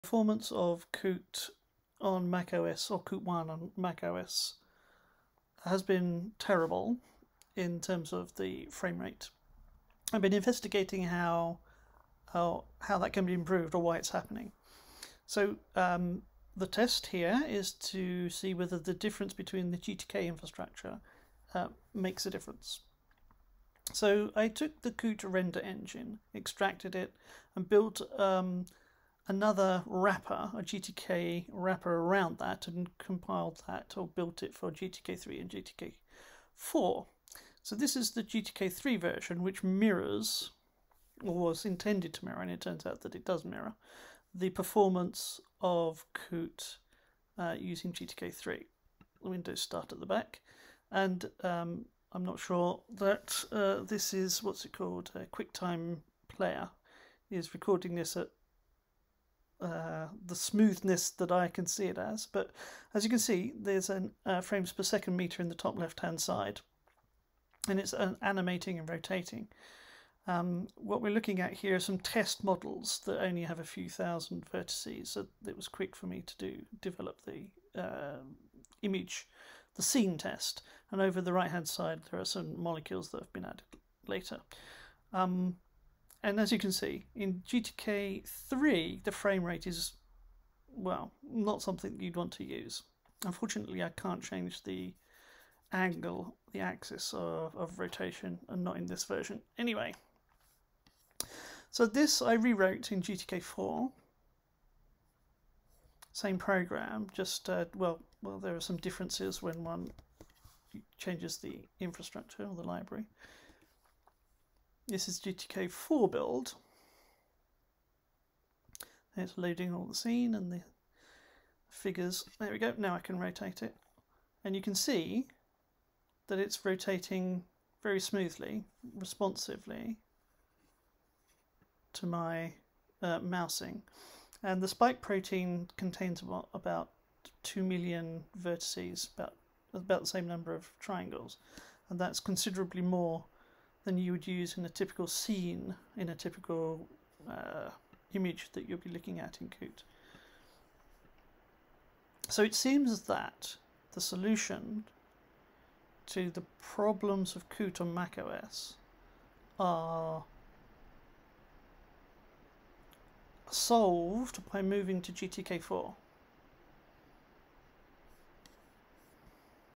Coot 1 Render Engine on Mac OS: Gtk3 vs Gtk4
How does Gtk4 compare against Gtk3 on Mac OS for OpenGL?

I don't know what the capture rate of QuickTime Player is, but it seems that it doesn't capture the full smoothness of the GTK4 version.